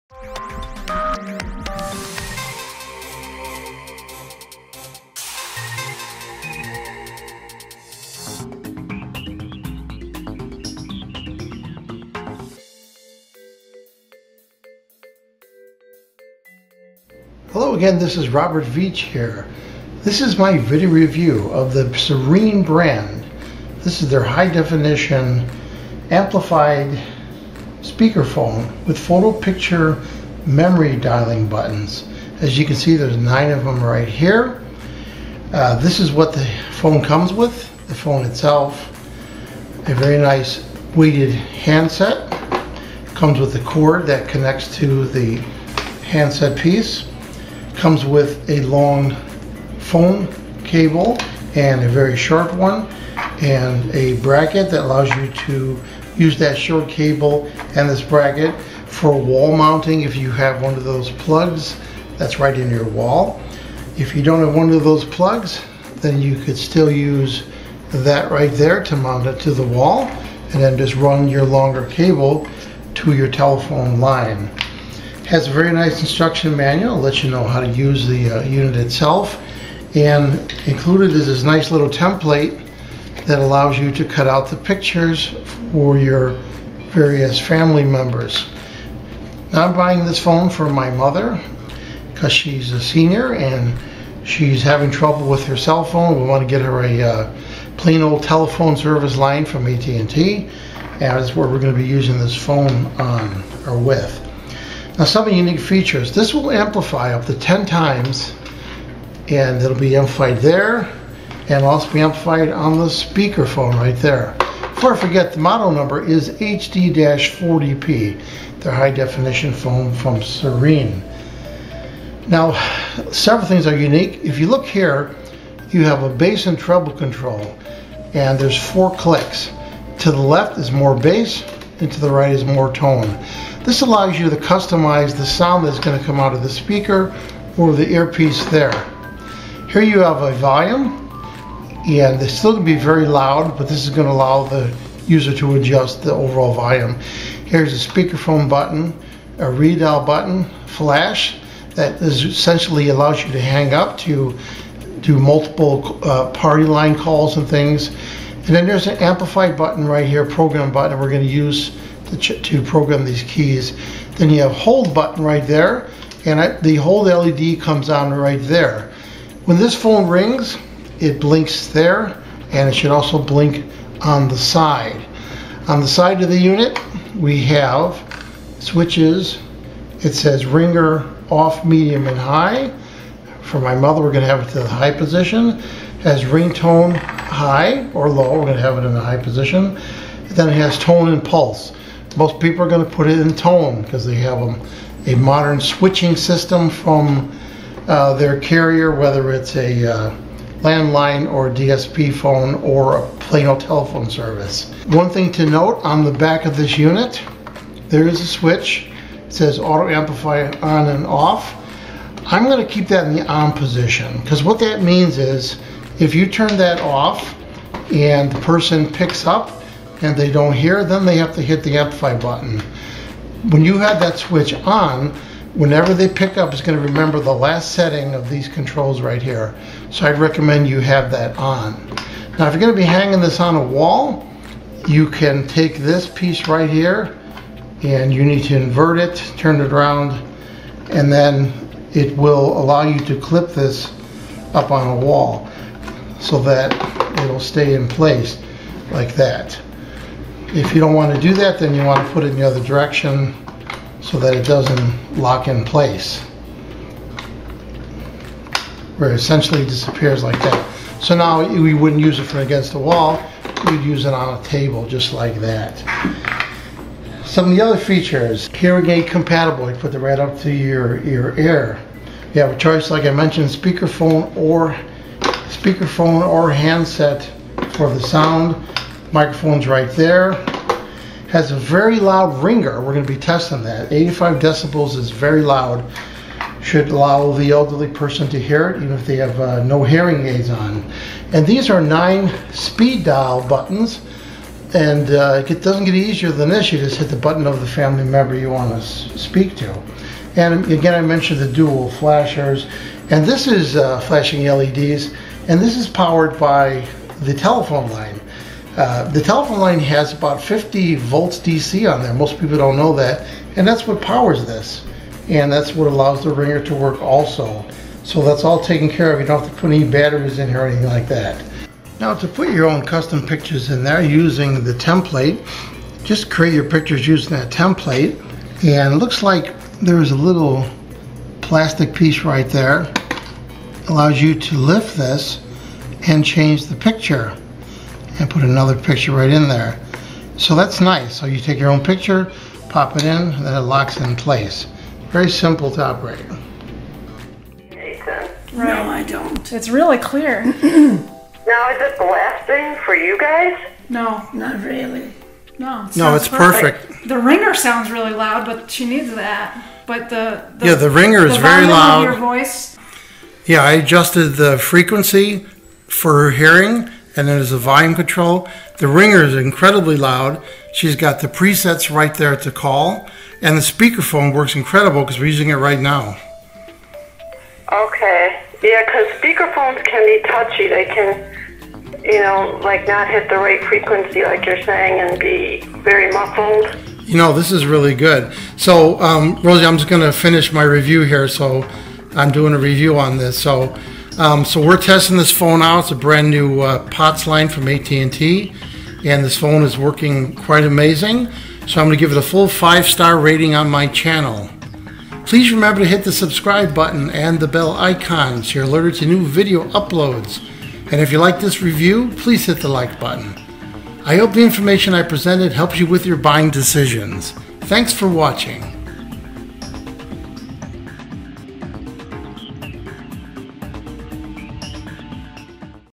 HIGH QUALITY Amplified Photo Dial Speakerphone for Hearing-Memory Loss Serene 26dB REVIEW
ORDER the Serene 26dB Amplified Photo Dial Speakerphone for Hearing-Memory Loss

** THIS PHONE is a Must Have for people with mild to moderate hearing loss and memory loss,  Amplifies incoming sound up to 26dB, 9 Photo Slots: Simply touch photo to dial number,  Built-in Speakerphone, Hearing-aid compatible.

□ Electronic Components
         □ Hobbies: